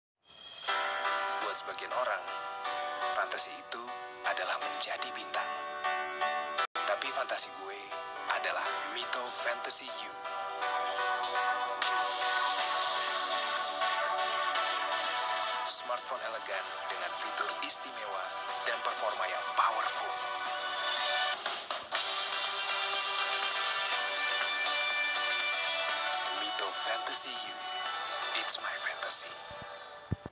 Iklan Mito Fantasy U - It's My Fantasy (2014) @ Indosiar, RCTI, SCTV, Trans 7, MNCTV, & Trans TV
Iklan ini pernah ditayangkan di Indosiar, RCTI, SCTV, Trans 7, MNCTV, & Trans TV pada tahun 2014.
Featuring: Al Ghasali